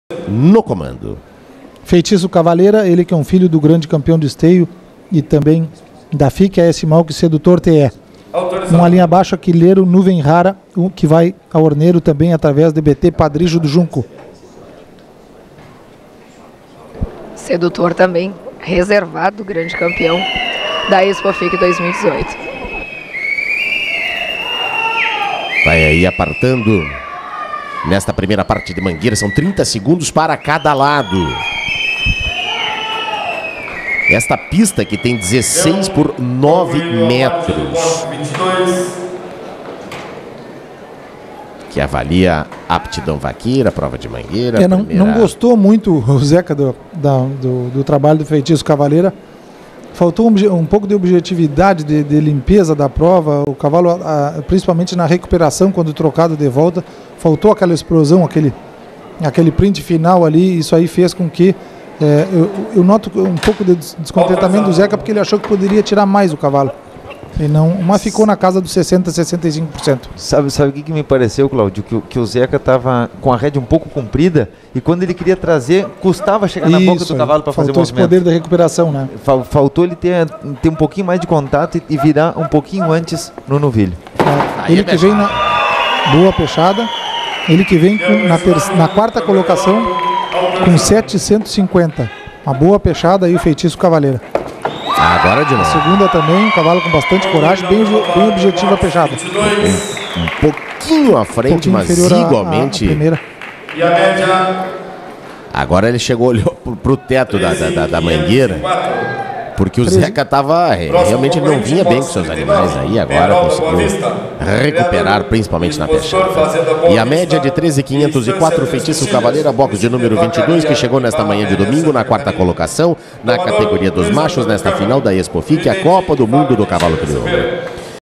FICCC de Esteio (RS) – 4° lugar machos – Feitiço Cavalera (Prova: Mangueira II)
Feitiço Cavalera conquistou o quarto lugar na prova da Mangueira II na categoria macho, com 20,025 pontos.